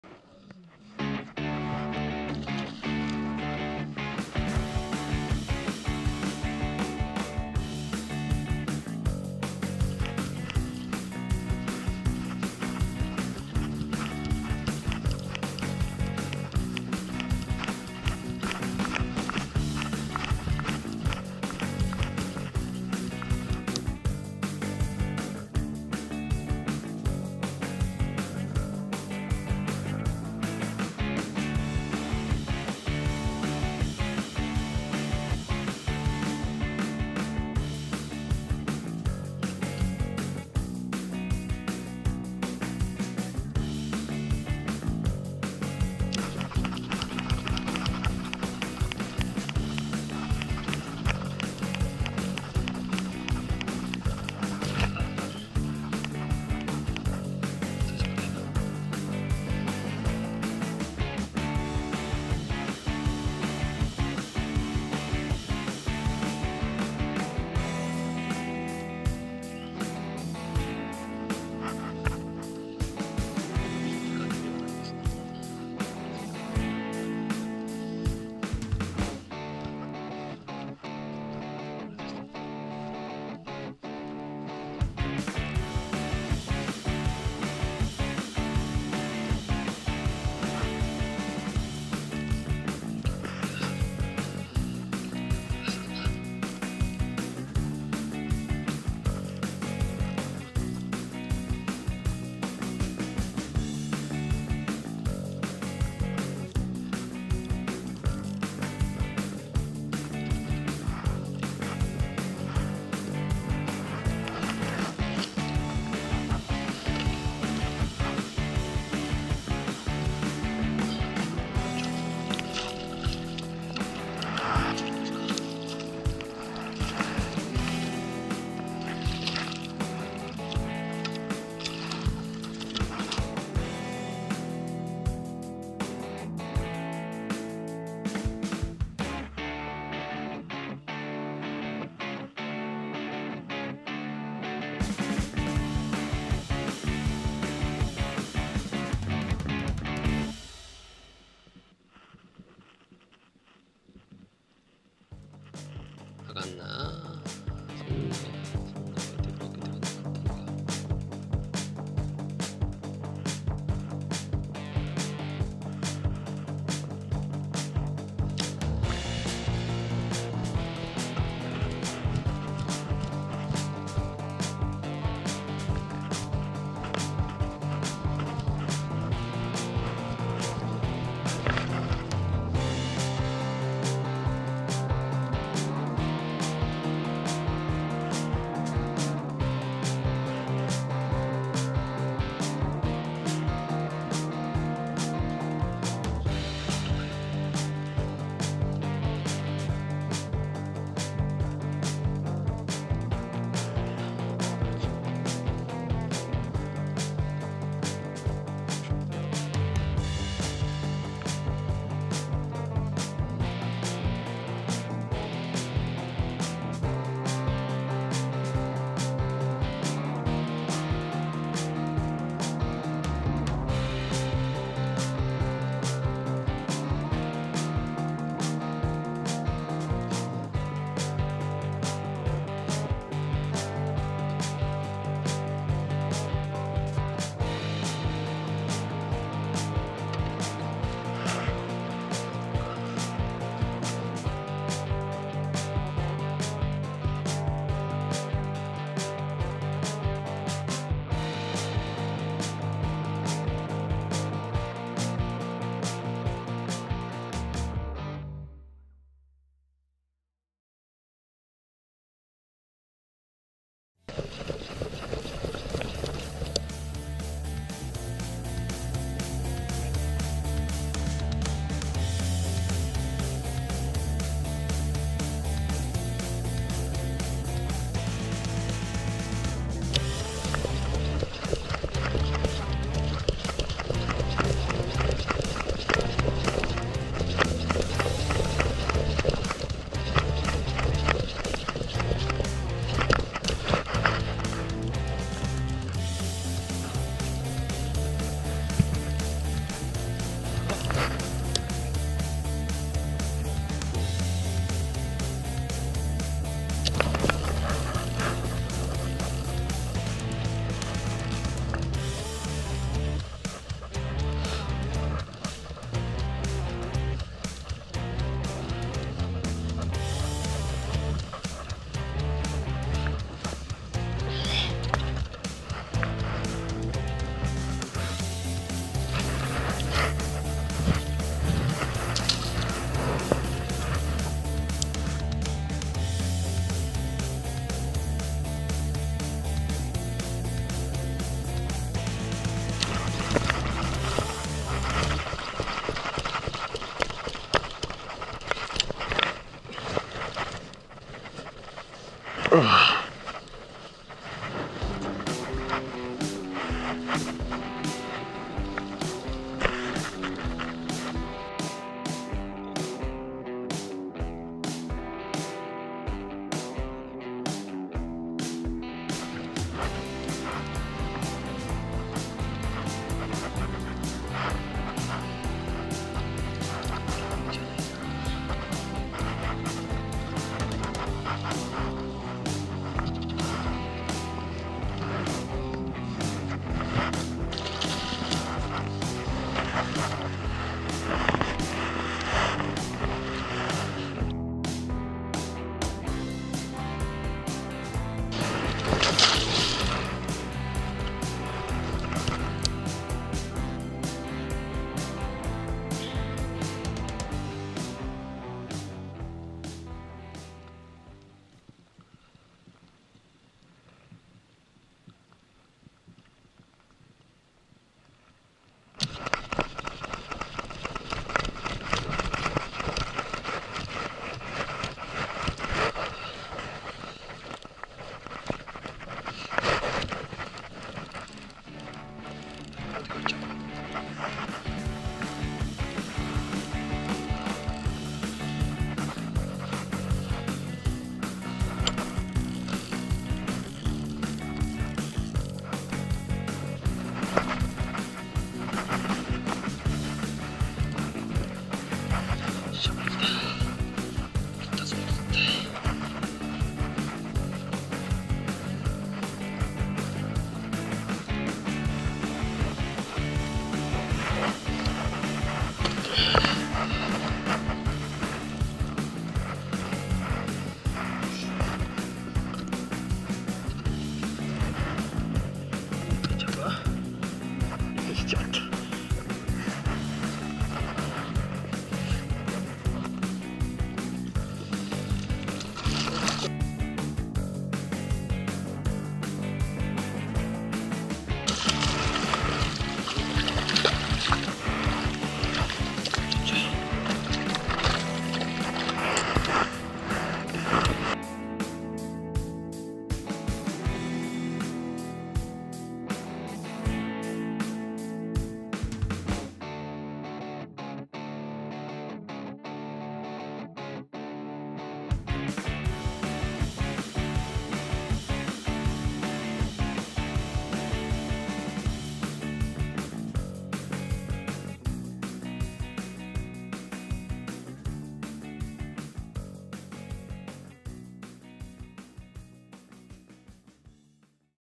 2024年春 青物シーズン開幕か！？
4月も半ばになり
そろそろ青物の便りも聞かれるように
近場のポイントからジギングで狙います
幸先よくいきなりヒット！！